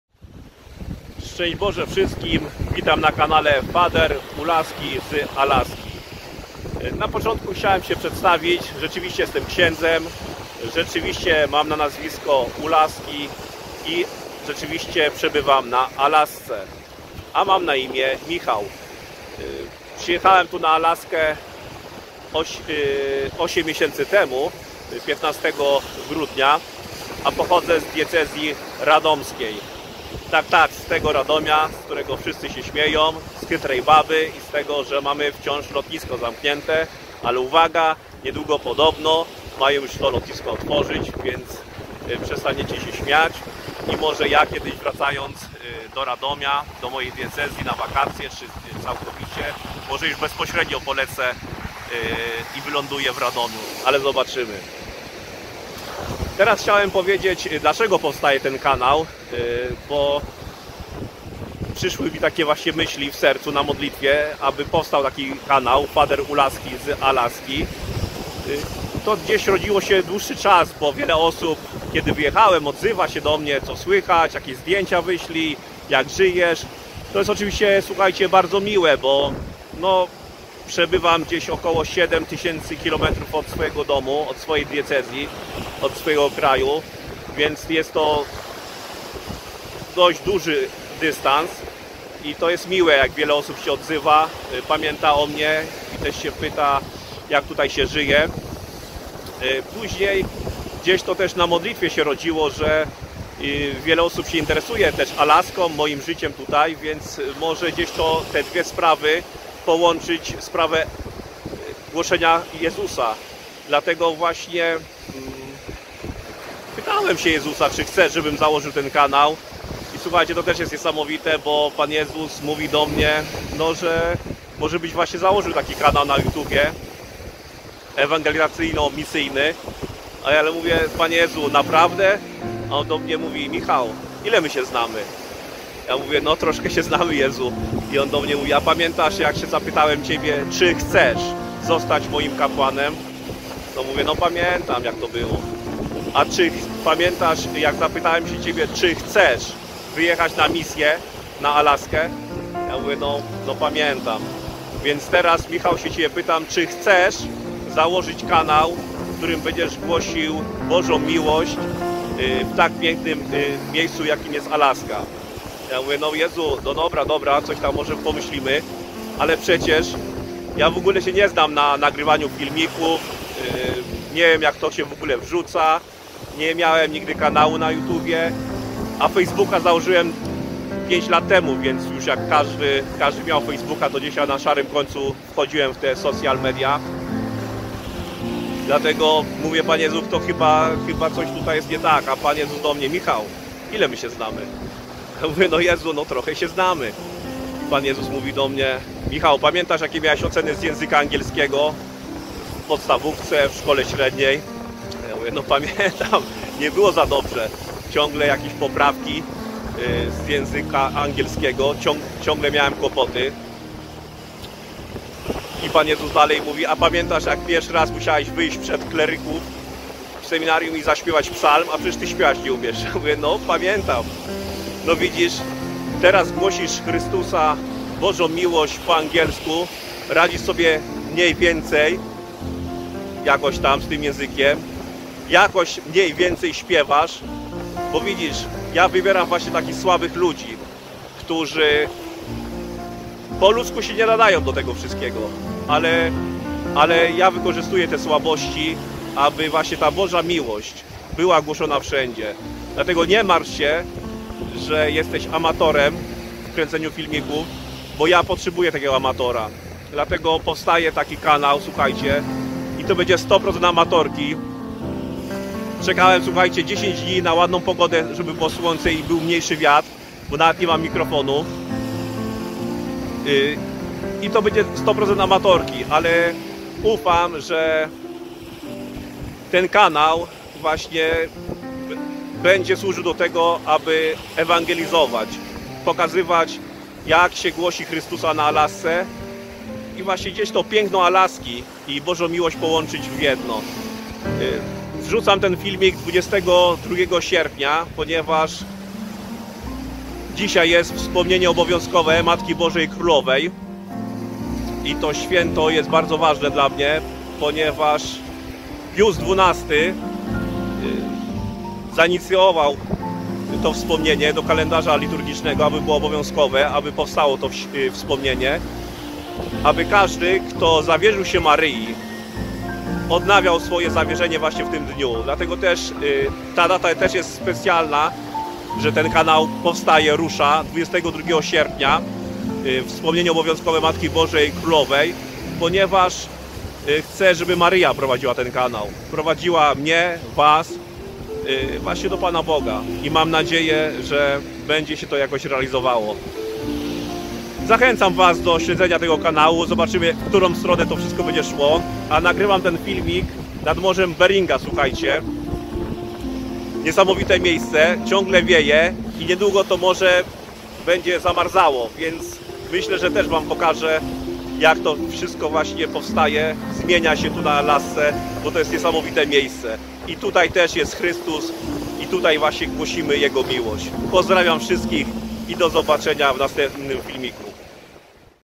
Father Ulaski z Alaski #1
Film przedstawia kim jestem i dlaczego powstaje kanał na YT.
Montaż: Fr Piotr Oprych